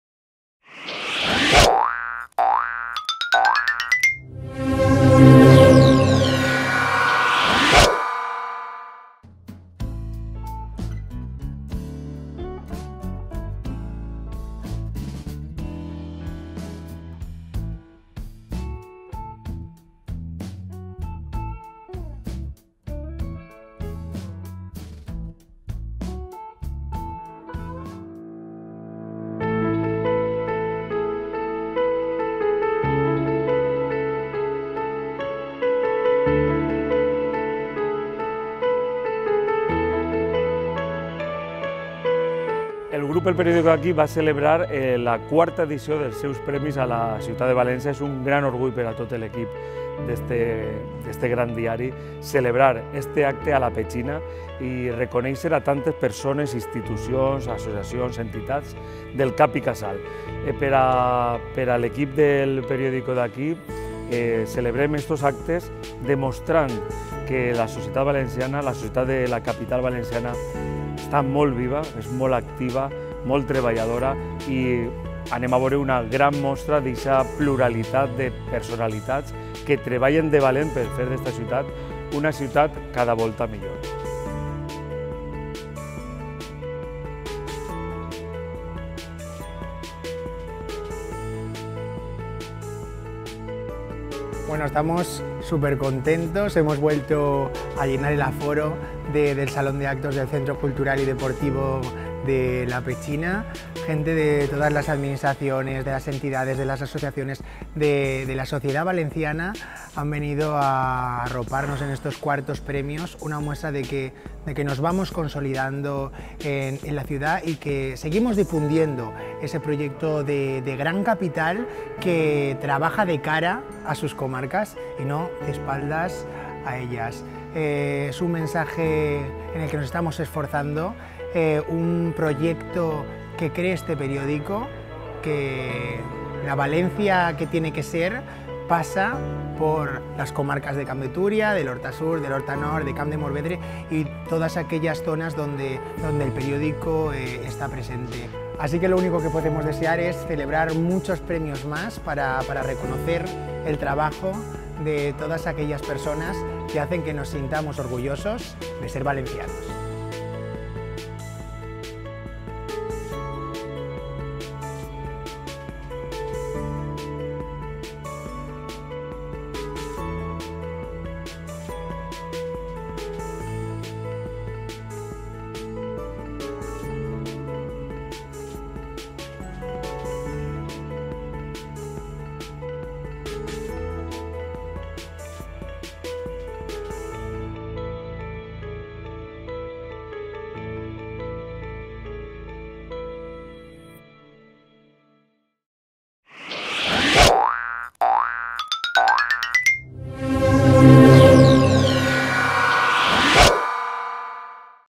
IV Premios de Valencia de El Periódico de Aquí
Vídeo de la gala de los IV Premios de El Periódico de Aquí de Valencia celebrada en el Centro Deportivo y Cultural de La Petxina.